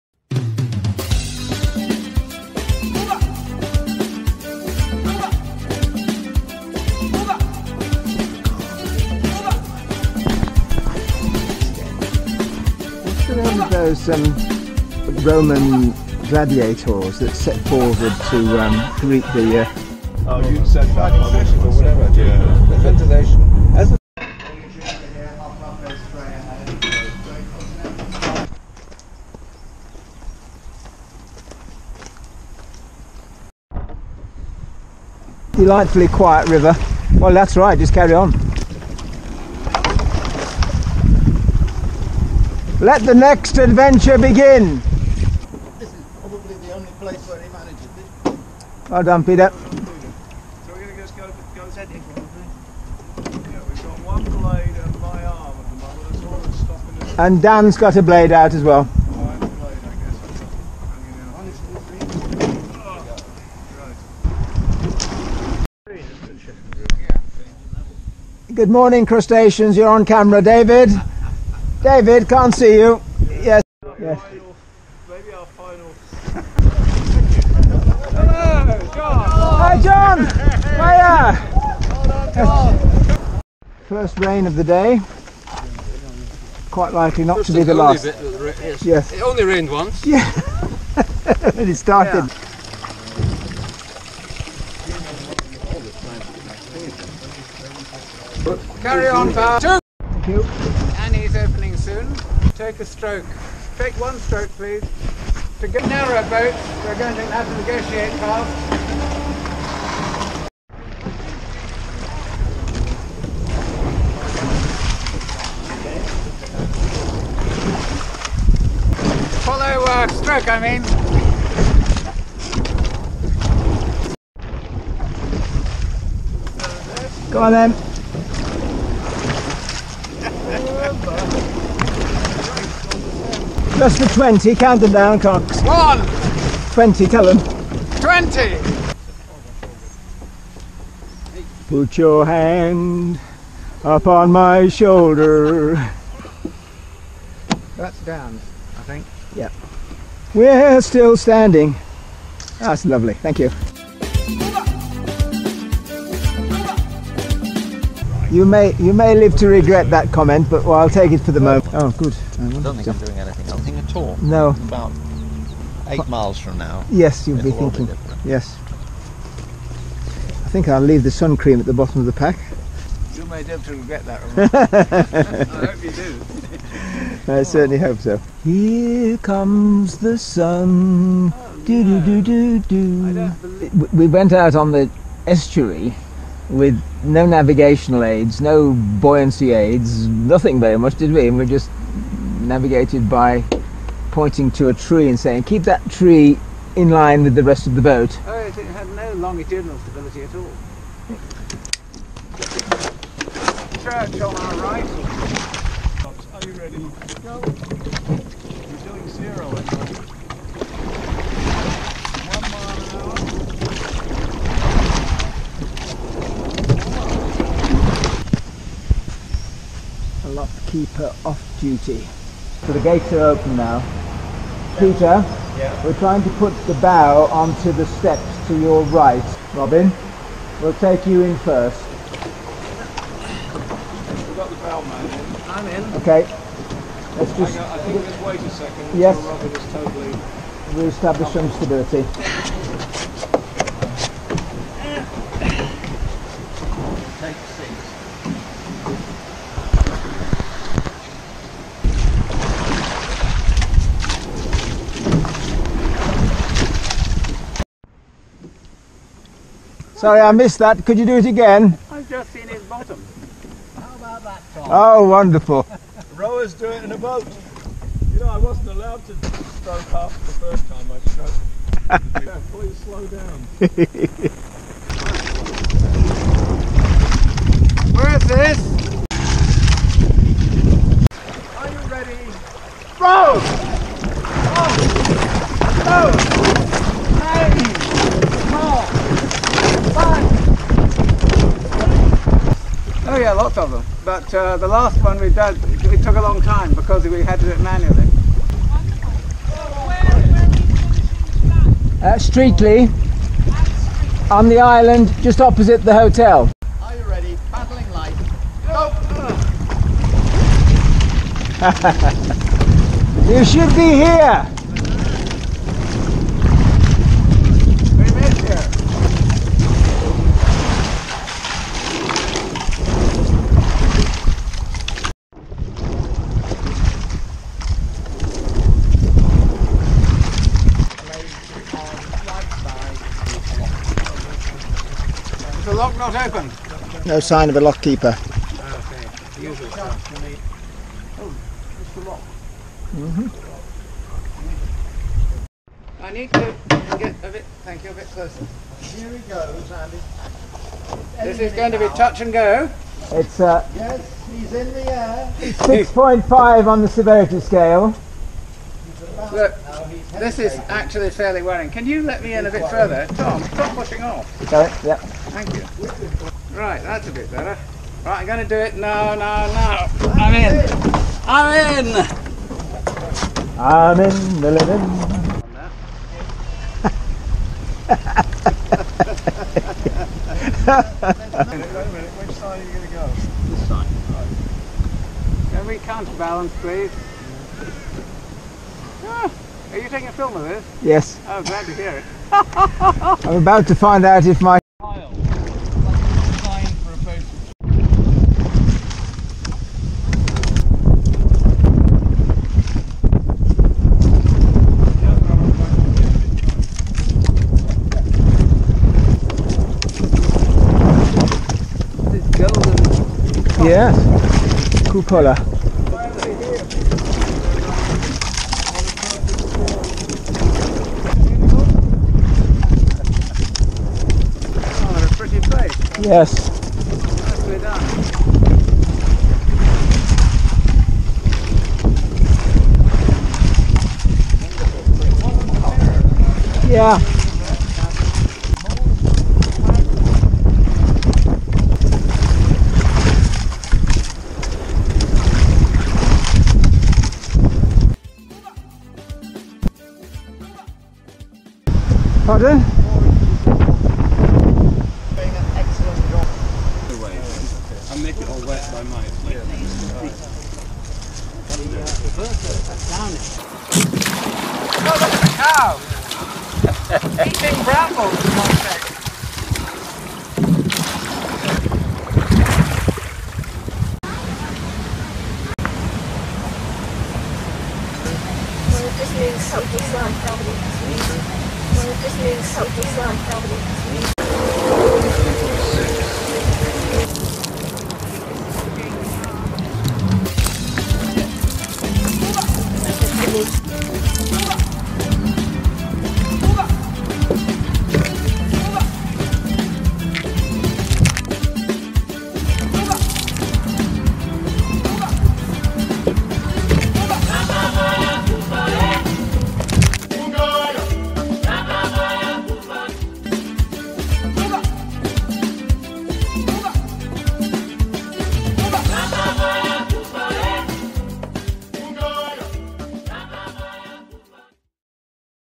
Crustaceans Mid Thames Oxford to Henley July 2017 Short version 8:45
5 Crustaceans  (alumni of Christs's College Boat Club, Cambridge) scull 46 miles in 2 days raising funds for Tasso Island Ecotourism Camp, Sierra Leone